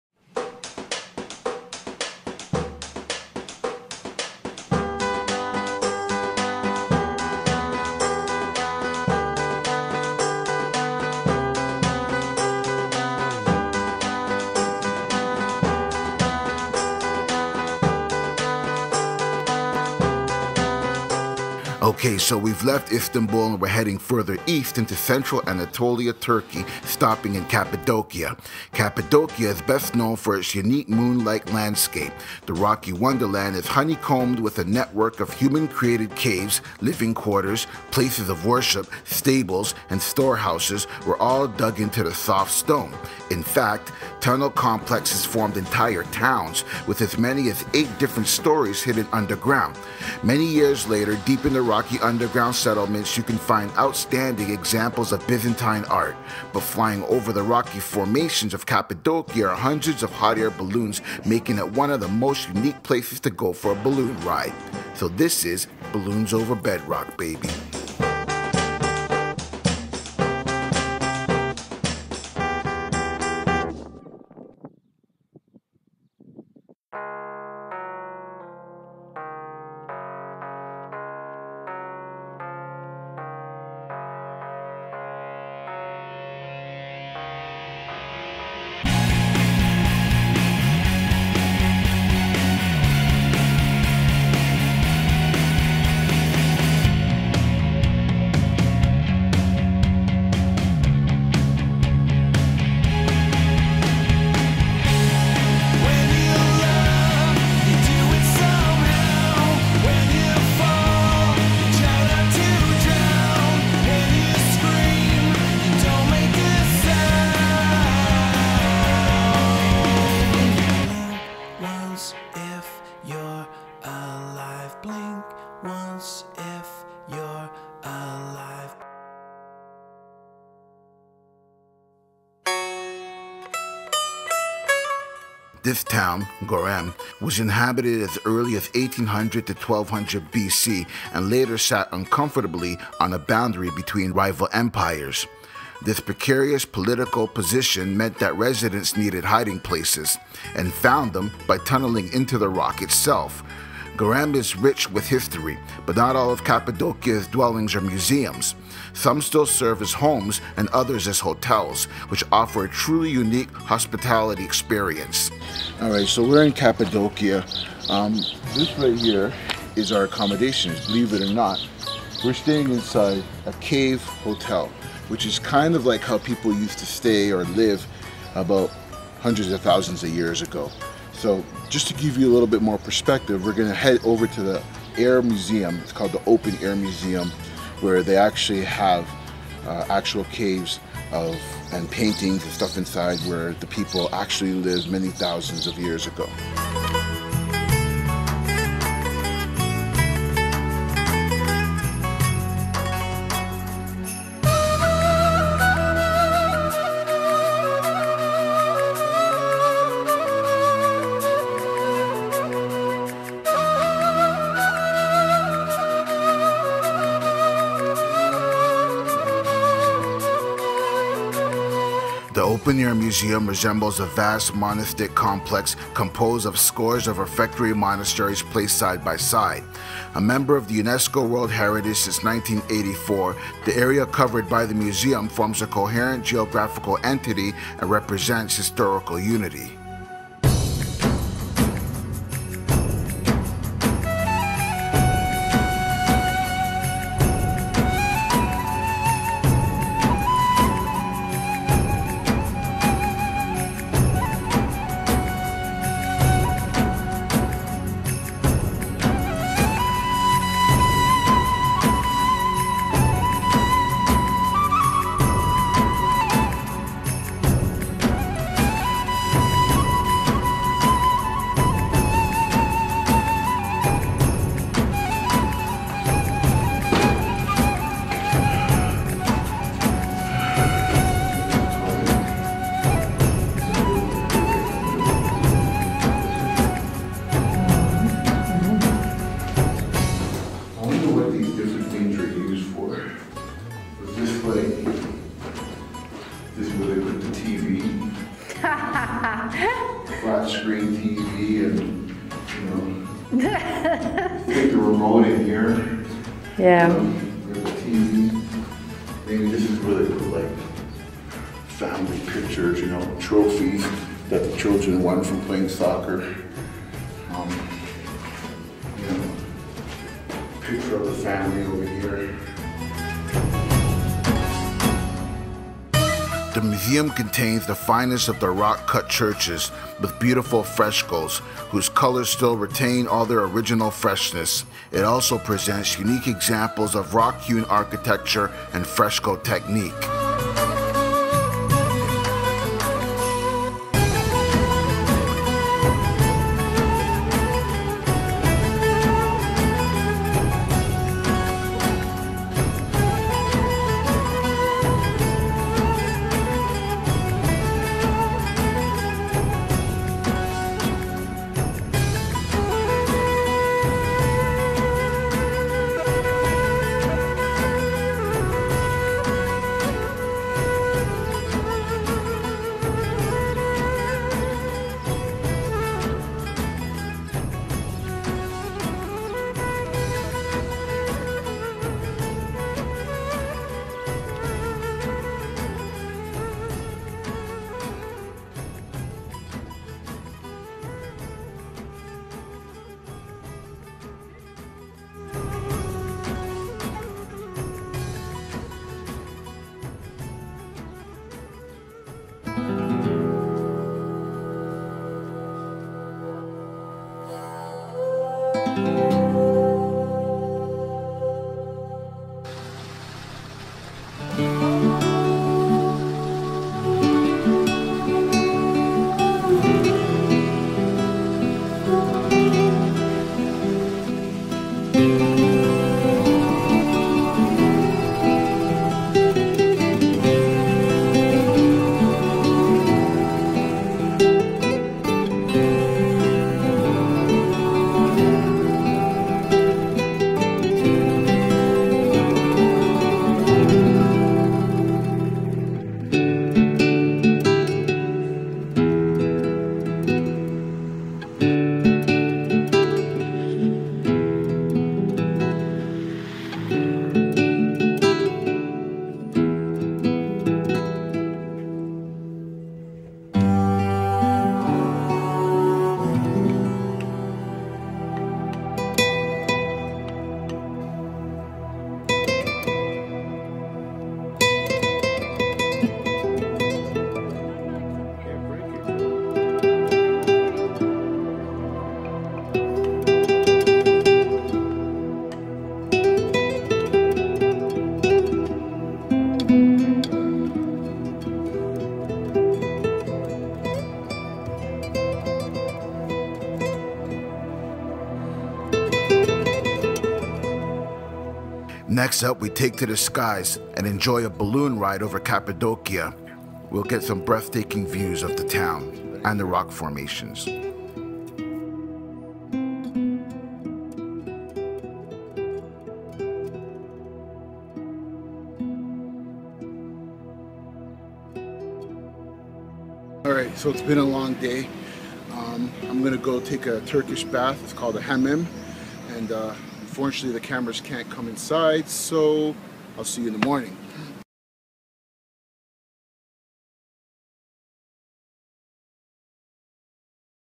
48 Hour Cappadocia Tour (Part 1)
With its underground cities, houses and cave churches in the rocks, it is true that Cappadocia in 48 hours will be a whirlwind of amazement and wonder. For instance, the Goreme Valley was shaped by the previous volcanic eruptions that once have eroded.

Layover:
Cappadocia in 48 Hours:

Equipment Used:
iPhone 6: